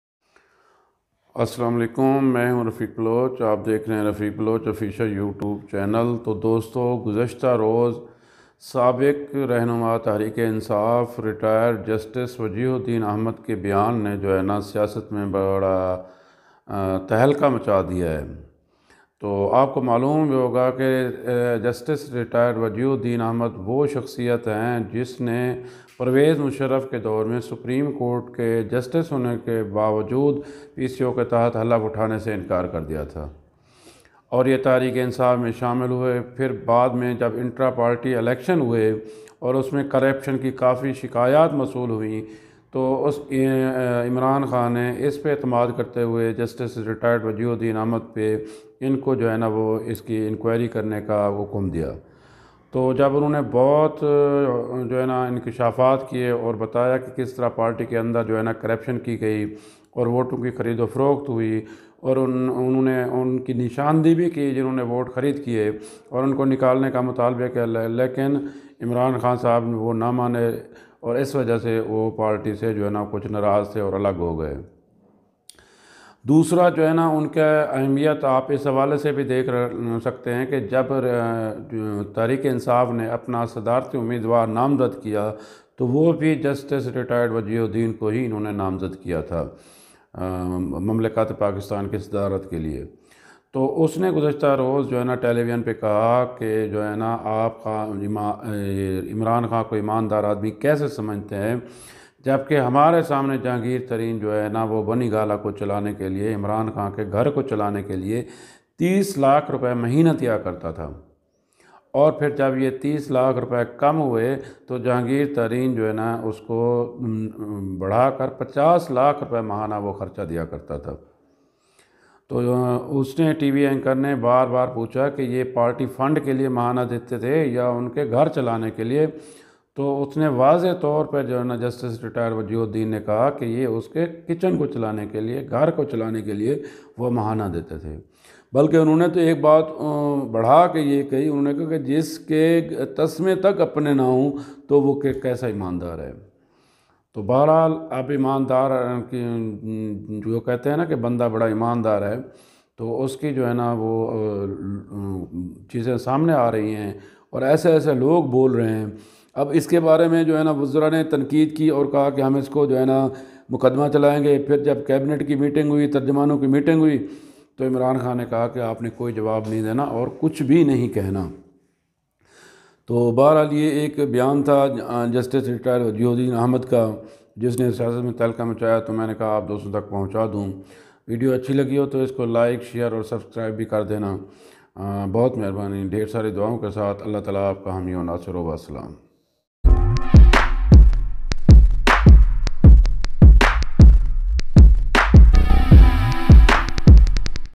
Breaking news .جہانگیر ترین عمران خان کےگھریلو اخراجات کے لے پچاس لاکھ ماہانہ دیتے تھےہ
عمران خان کےگھریلو اخراجات کے لے پچاس لاکھ ماہانہ دیتے تھےہ